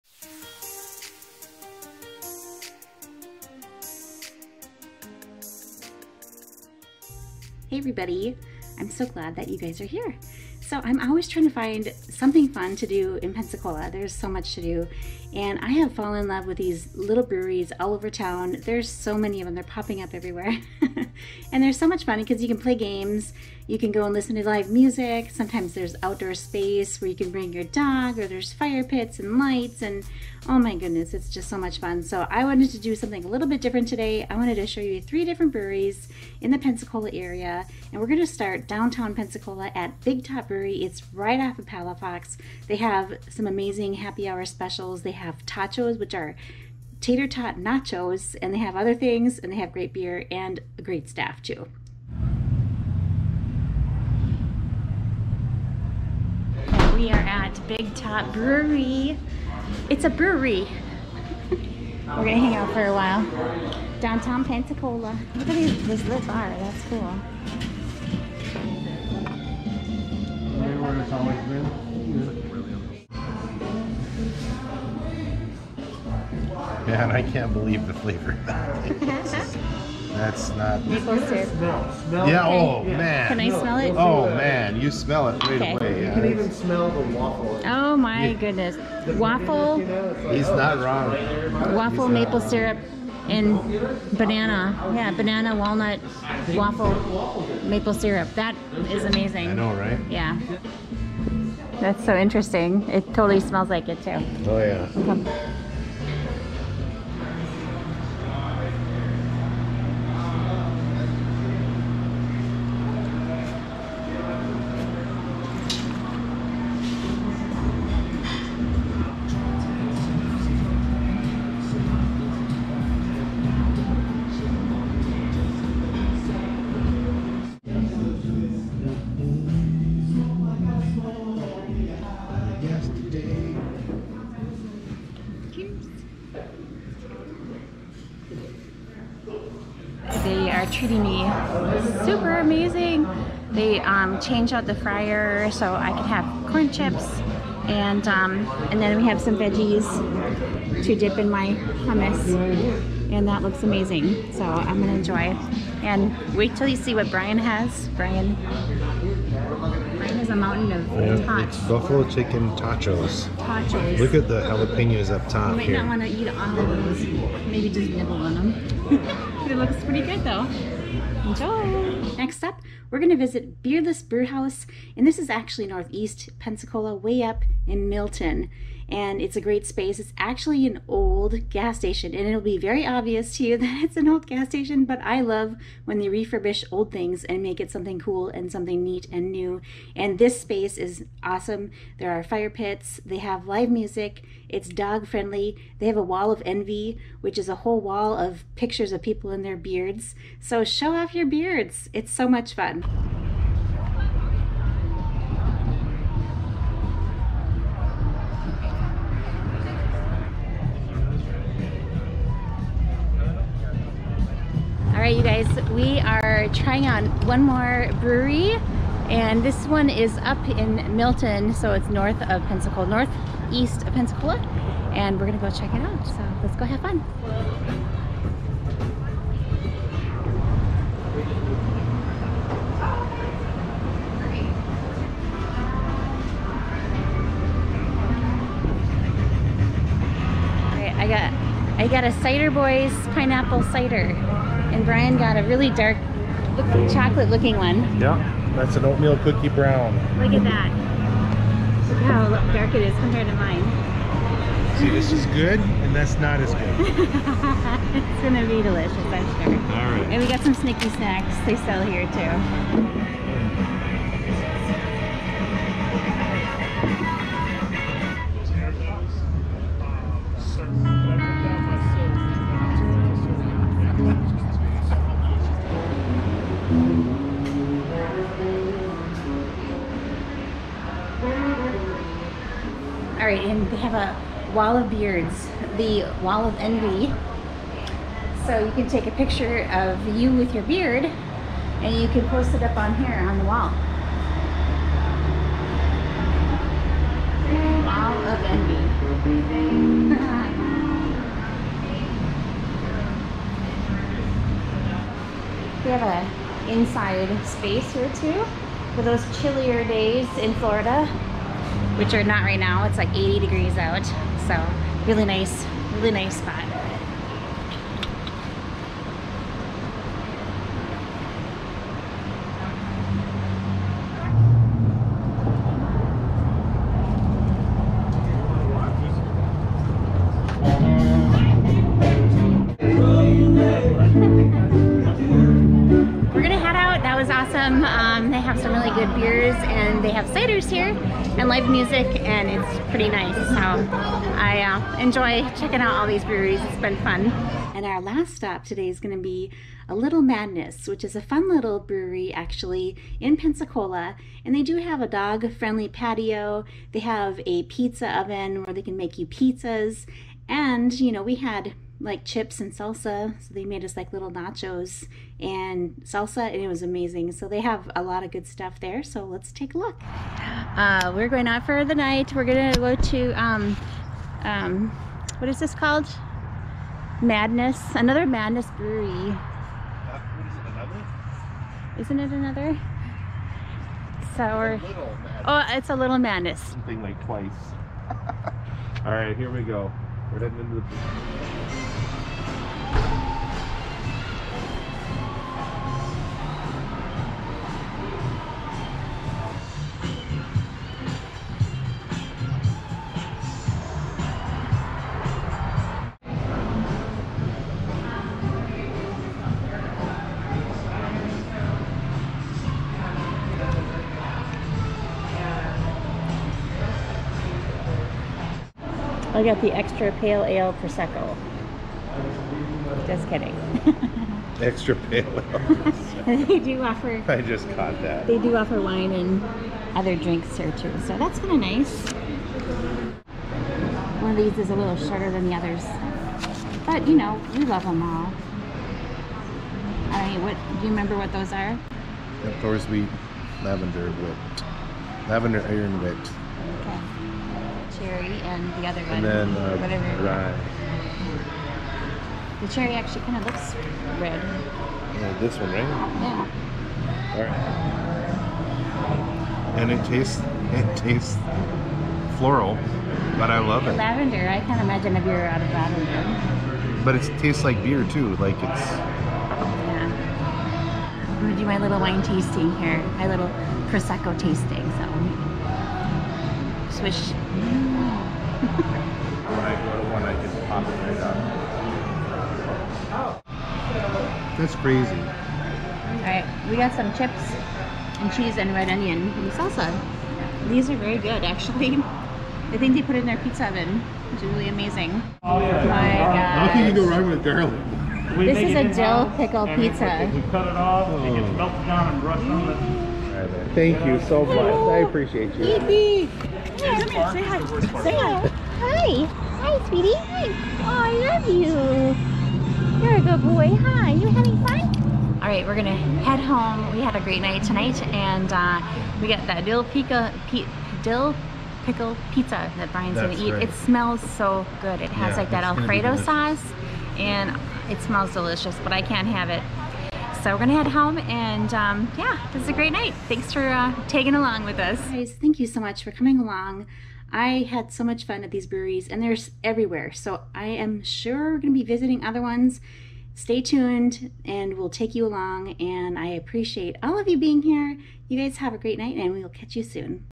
Visiting 3 Pensacola Breweries - Things to do in Pensacola! Big Top | Beardless | A Little Madness
I am always looking for fun things to do in Pensacola.  I have fallen in love with Breweries with local draft beers and fun things to do and see. They have popped up all over the place.  In this video we will be checking out Big Top Brewery downtown Pensacola, Beardless Brewhaus up in MIlton which is a refurbished gas station with a cool outdoor vibe and A Little Madness in Pensacola with dog friendly patio and pizza oven with unique pizzas like Dill Pickle Pizza!  These are all a fun way to spend a date night or group hangout!

Connect with Dancer Quest!
We would love to have you along!
Pinterest DancerQuest
Ship Mate Dancer Quest
Dancerquest.com